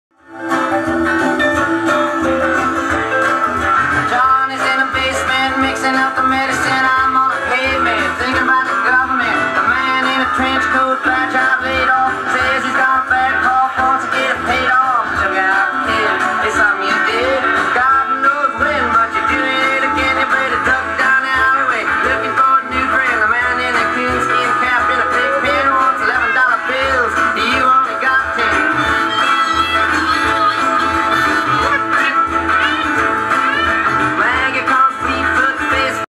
Alexander Dancing
Alexander jamming out to some Bob Dylan